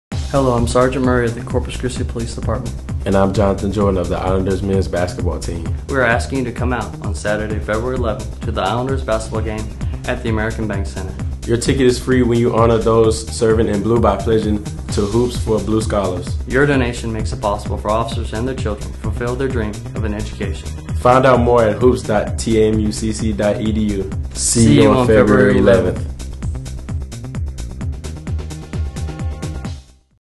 TAMUCC Support 'Hoops For Blue Scholars' (2012 Promo)
- Police officers put their lives on the line for the community every day, yet most don't receive financial aid to fund a college education for themselves or their family members. The Hoops for Blue Scholars fundraiser builds the endowment for the One Hundred Club Scholarship created in 1998 to support the Corpus Christi Police Department. Your gift directly benefits Corpus Christi police officers and their family members who attend Texas A&M University-Corpus Christi.

By pledging so many dollars for every field goal (2- and 3-pointers) the Islanders Men's Basketball team scores against the Sam Houston State Bearkats at the American Bank Center on Feb. 11, you'll be helping our men and women in blue. You'll also receive a *free ticket to the big game, with the chance to purchase additional tickets at a discount!